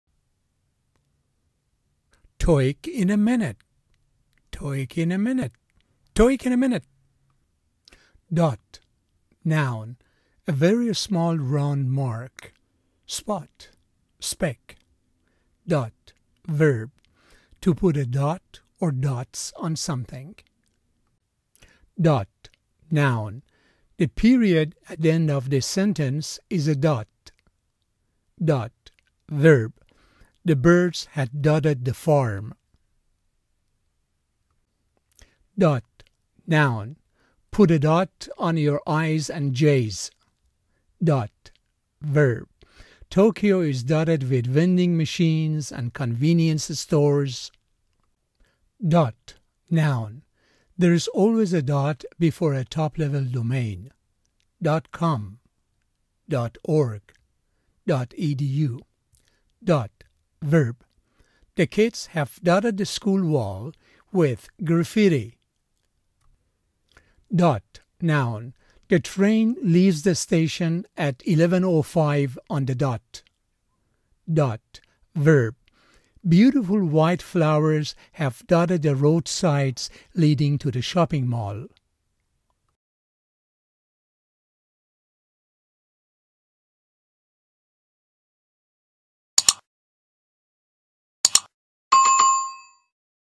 dot
The term "dot" is defined, pronounced, and used in a few examples.